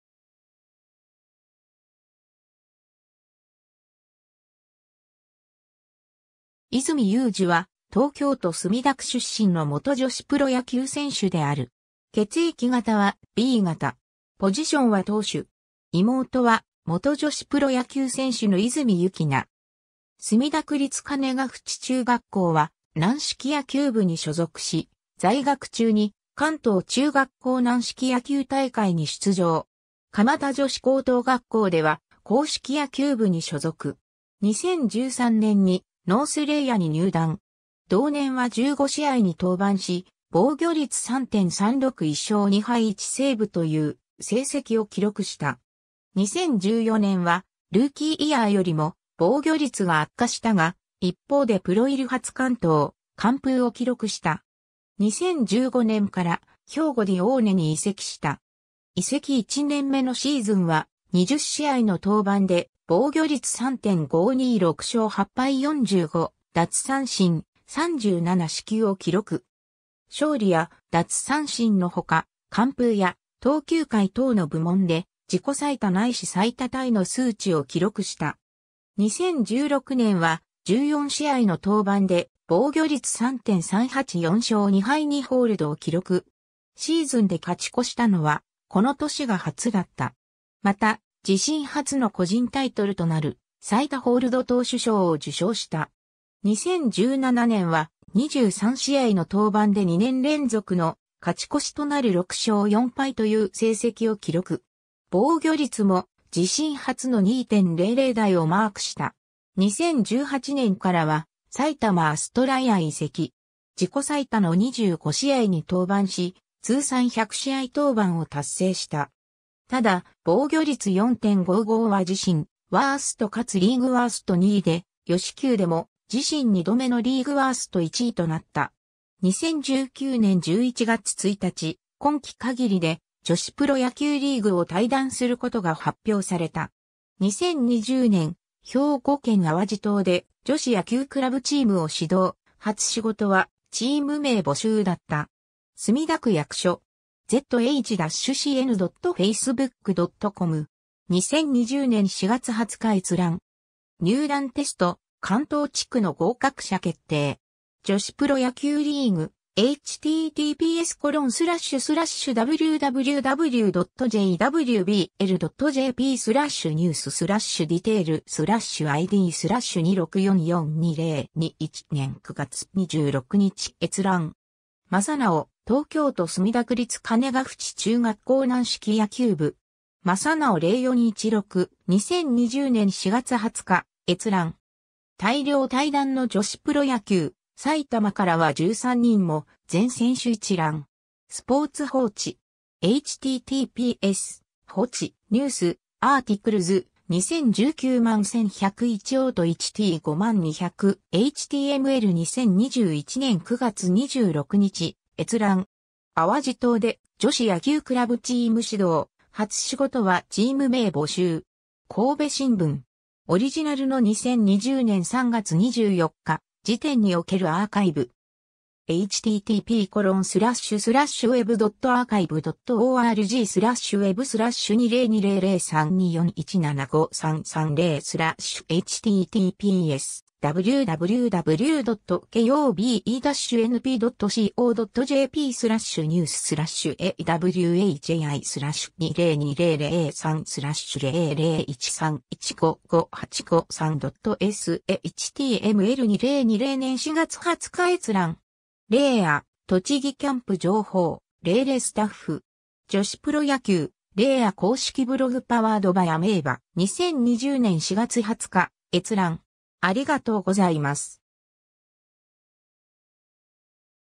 泉由有樹
泉 由有樹（いずみゆうき、1994年10月10日 - ）は東京都墨田区出身の元女子プロ野球選手である。
ポジションは投手。
 妹は元女子プロ野球選手の泉由希菜。
 墨田区立鐘淵中学校は軟式野球部に所属し、在学中に関東中学校軟式野球大会に出場。
 蒲田女子高等学校では、硬式野球部に所属。
 2013年に、ノース・レイアに入団。
同年は15試合に登板し、防御率3.36・1勝2敗1セーブという成績を記録した。
 2014年はルーキーイヤーよりも防御率が悪化したが、一方でプロ入り初完投・完封を記録した。
 2015年から、兵庫ディオーネに移籍した。
移籍1年目のシーズンは、20試合の登板で防御率3.52・6勝8敗・45奪三振・37四球を記録。
勝利や奪三振の他、完封や投球回等の部門で、自己最多ないし最多タイ (2018年度シーズン終了時点) の数値を記録した。
 2016年は、14試合の登板で防御率3.38・4勝2敗2ホールドを記録。
シーズンで勝ち越したのは、この年が初だった。
また、自身初の個人タイトルとなる最多ホールド投手賞を受賞した。
 2017年は、23試合の登板で2年連続の勝ち越しとなる6勝4敗という成績を記録。
防御率も自身初の2.00台 (2.70) をマークした。
 2018年からは埼玉アストライアへ移籍。
自己最多の25試合に登板し、通算100試合登板を達成した。
ただ、防御率4.55は自身ワースト且つリーグワースト2位で、与四球でも自身2度目のリーグワースト1位となった。
 2019年11月1日、今季限りで女子プロ野球リーグを退団することが発表された。
 2020年、兵庫県淡路島で女子野球クラブチームを始動、初仕事は「チーム名募集」だった。
 ^ “墨田区役所” (中国語). zh-cn.facebook.com. 2020年4月20日閲覧。
^ “入団テスト関東地区の合格者決定！！” (プレスリリース), 女子プロ野球リーグ, (2014年10月16日), 2021年9月26日閲覧。
 ^ Masanao (2017年9月19日). “東京都墨田区立鐘淵中学校軟式野球部(鐘ヶ淵イーグ” (日本語). @MASANAO_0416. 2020年4月20日閲覧。
^ “大量退団の女子プロ野球、埼玉からは13人も…全選手一覧”. スポーツ報知. (2019年11月1日). 2021年9月26日閲覧。
 ^ “淡路島で女子野球クラブチーム始動　“初仕事”はチーム名募集”. 神戸新聞. (2020年3月1日). オリジナルの2020年3月24日時点におけるアーカイブ。
2020年4月20日閲覧。
^ “レイア『栃木キャンプ情報☆【00スタッフ】』” (日本語). 女子プロ野球 レイア公式ブログ Powered by Ameba. 2020年4月20日閲覧。